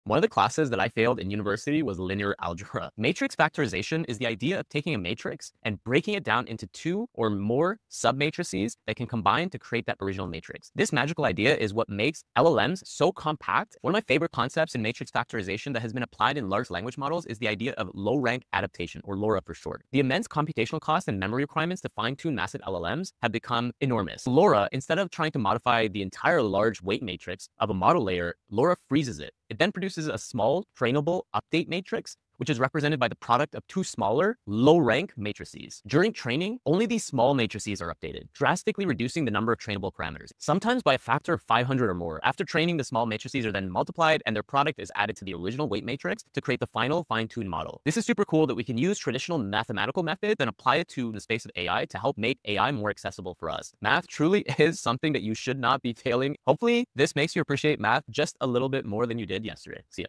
Low Rank Adaptation (LoRA)
I'm not ashamed to admit I failed linear algebra in university. 😬 But now, one of its core concepts is my favorite thing in AI. Let's talk about LoRA (Low-Rank Adaptation).

The problem: Fine-tuning massive LLMs requires enormous computational power and memory.

The solution: Instead of retraining every parameter in a huge model, LoRA freezes the original model. It then injects a pair of small, "low-rank" matrices that are trained instead. After training, these small matrices are simply added back to the original.

This simple idea, rooted in matrix factorization, drastically reduces the number of trainable parameters (sometimes by a factor of 500 or more!), making it cheaper and easier for more people to build custom AI. It’s a perfect example of how classic mathematical methods are making modern AI more accessible. 🧠✨

Maybe it's time to appreciate math a little bit more.